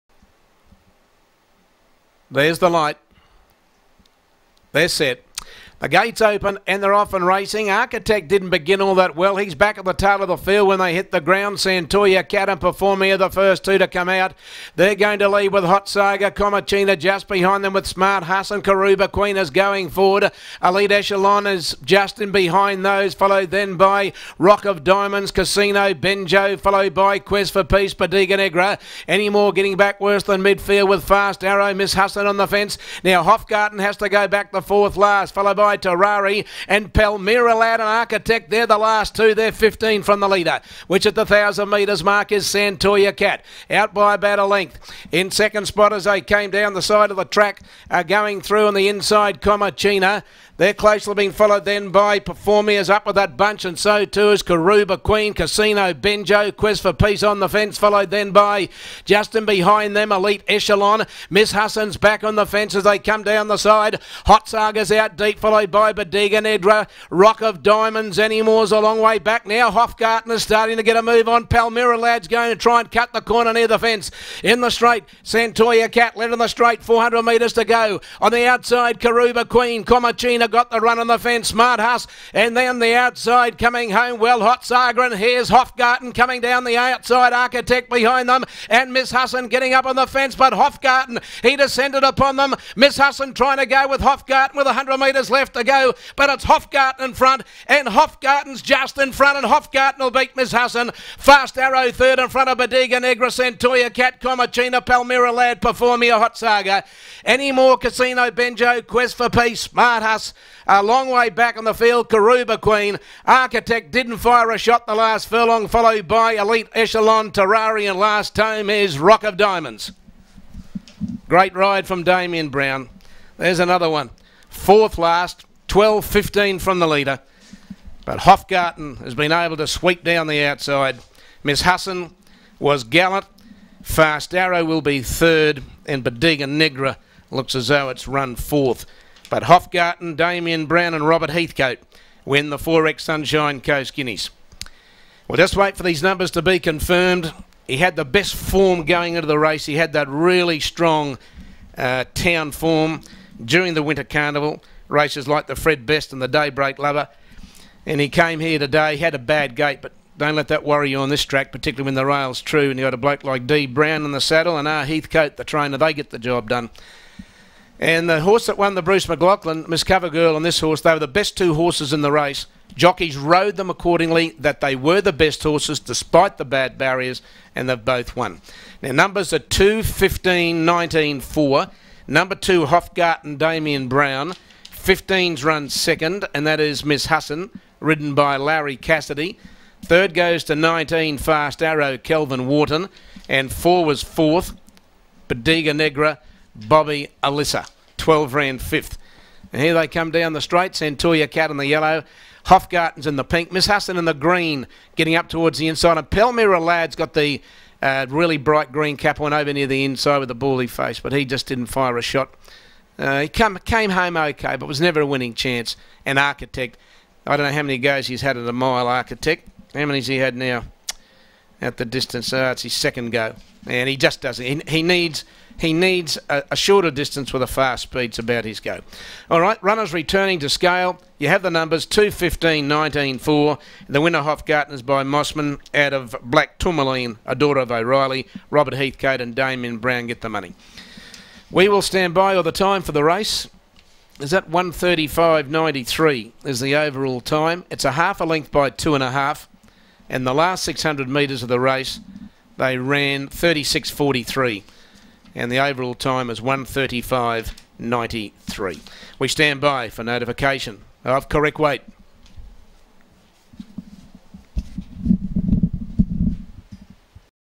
Hopfgarten - Win at Caloundra 28 June 14' (Group 3)
Hopfgarten - Win at Caloundra 28 June 14' (Group 3). Jockey: Damian Browne. Colours: Black with Pink Checks and Black with Pink Checks Cap.